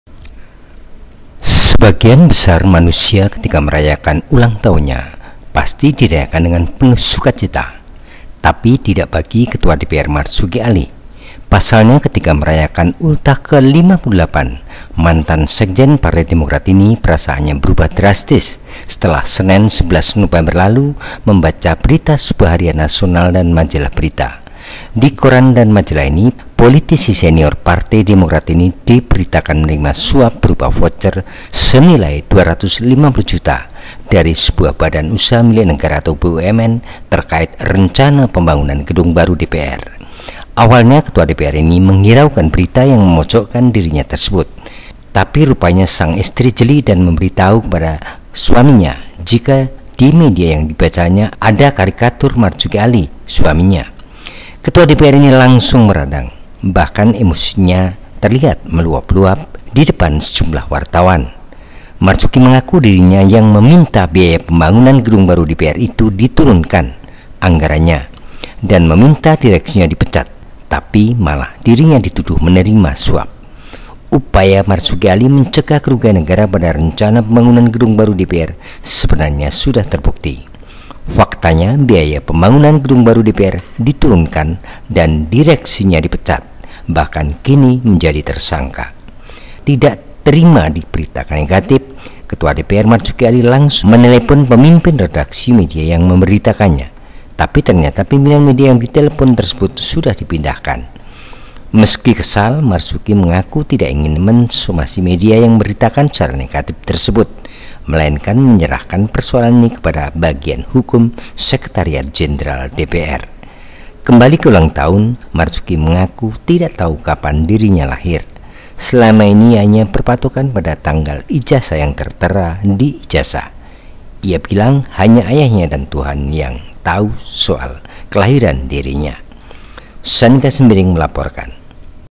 Marzuki Alie Tak Ingin Somasi Media yang Memberitakan Negatif
Sebagian besar manusia ketika merayakan ulang tahunnya, dengan penuh sukacita. Tapi, tidak bagi Ketua DPR Marzuki Alie. Pasalnya, ketika merayakan Ultah ke-58, mantan Sekjen Partai Demokrat ini perasaannya berubah drastis, setelah Senin (11/11) lalu, membaca berita sebuah harian nasional dan majalah berita. Di koran dan majalah ini, politisi senior Partai Demokrat ini diberitakan menerima suap berupa voucher senilai Rp 250 juta dari sebuah Badan Usaha Milik Negara (BUMN), terkait rencana pembangunan gedung baru DPR.